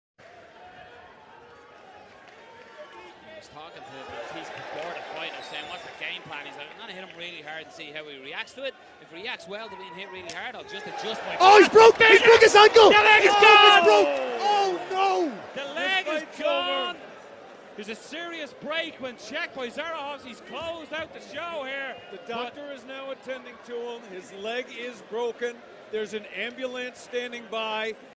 caposa A checked low kick by Matiss Zaharovs shatters Hussain Maki's leg at Brave CF 29 Thankfull
Matiss Zaharovs shatters Hussain Maki's leg at Brave CF 29